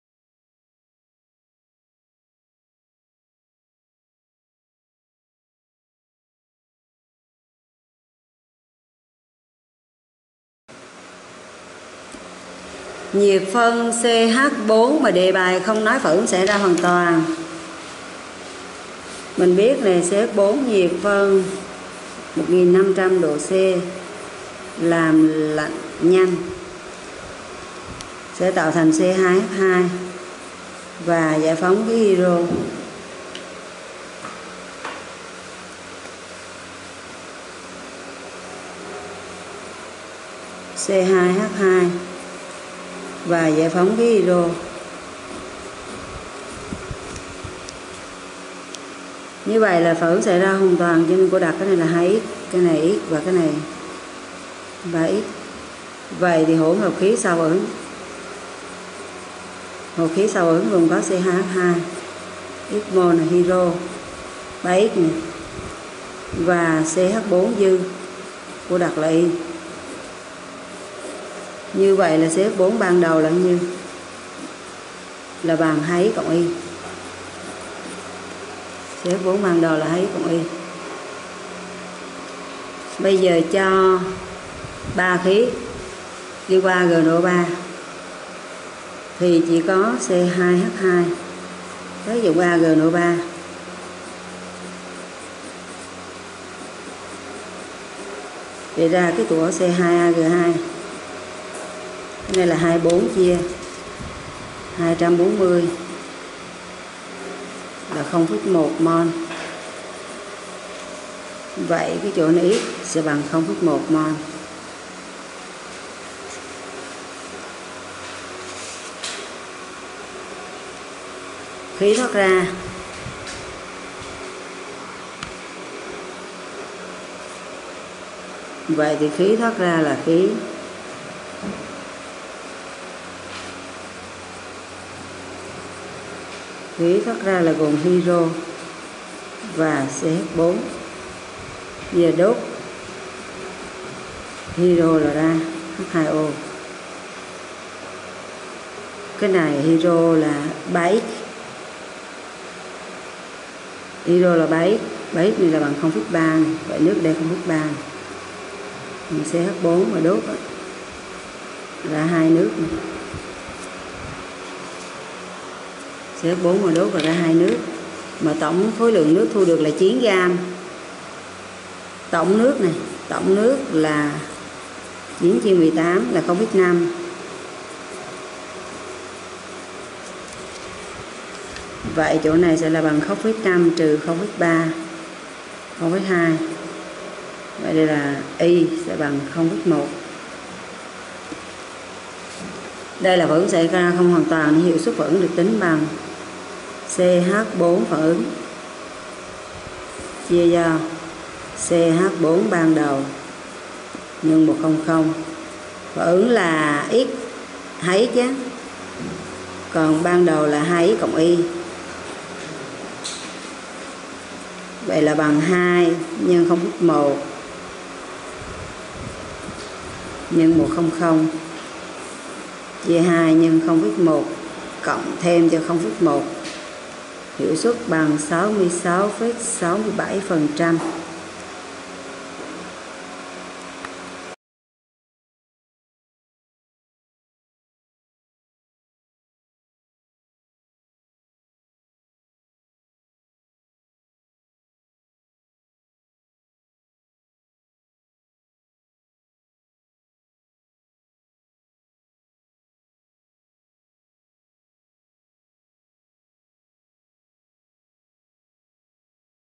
Nhiệt phân không hoàn toàn CH4
Metan khi đưa lên 1500oC rồi làm lạnh nhanh trong vòng 10-15s sẽ tạo thành acetilen và khí hidro.
Phản ứng này thông thường là phản ứng không hoàn toàn nên sau phản ứng metan còn dư.